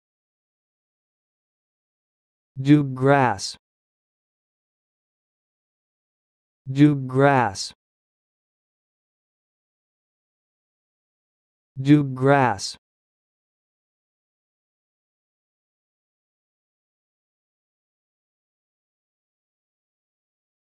Learn how to say this word: "Doob Grass"
This is the definition of "Doob Grass":
A perennial, creeping grass (Cynodon dactylon), highly
   prized, in Hindostan, as food for cattle, and acclimated in the United
   States.